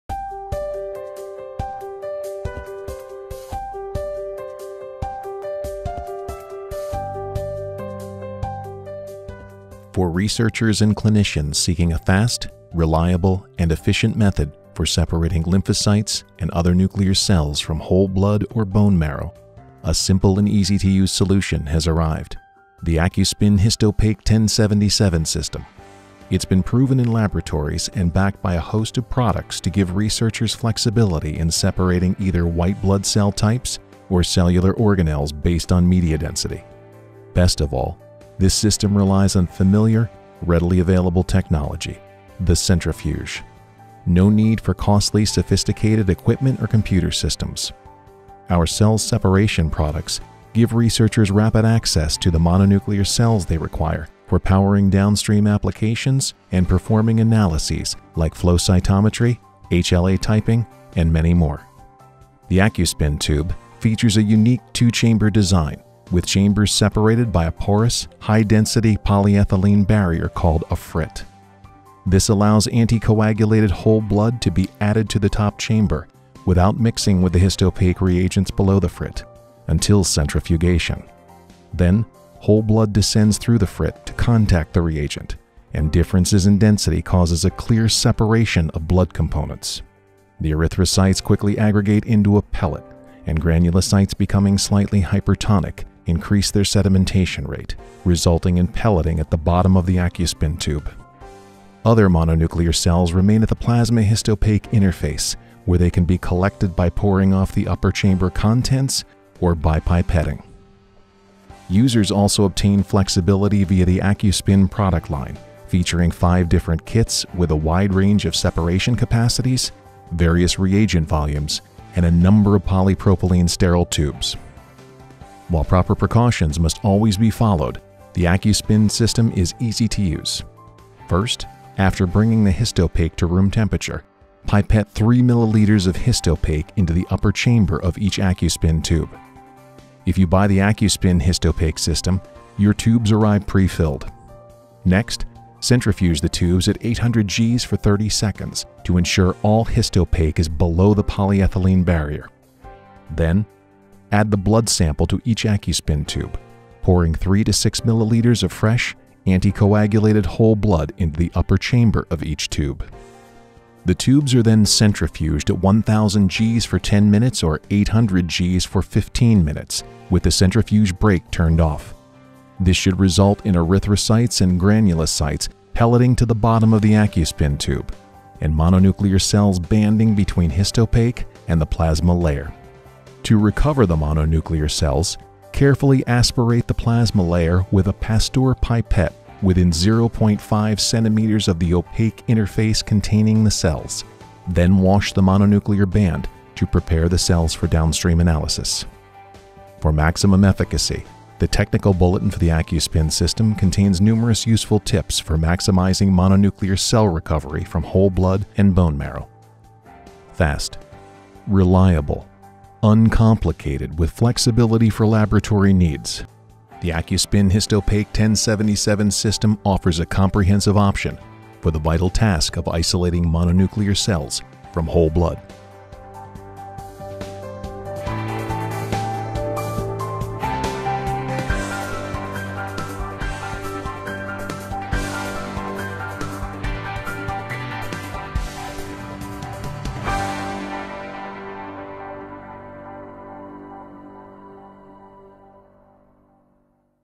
Accuspin™ System with Histopaque®-1077: Comprehensive Cell Separation
See how the Accuspin™ System with Histopaque®-1077 is an efficient method for separating lymphocytes and other nuclear cells from whole blood or bone marrow.  Our cell separation products give researchers rapid access to the mononuclear cells they require for powering downstream applications and performing analyses like flow cytometry, HLA typing, and many more